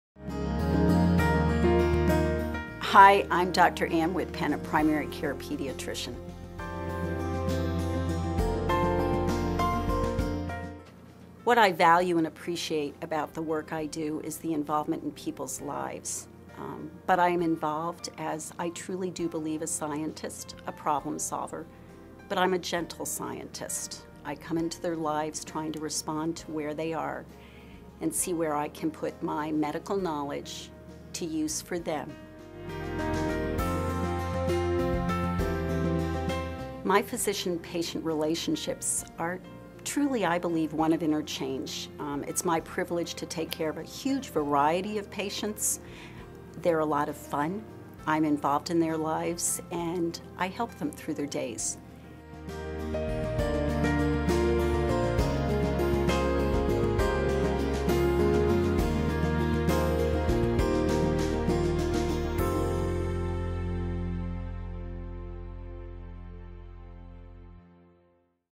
Ann S. Wittpenn, MD, Pediatrician - Williston VT, The UVM Medical Center
Dr. Wittpenn is a pediatrician at the University of Vermont Medical Center and an associate professor at the Larner College of Medicine at UVM in Burlington, VT.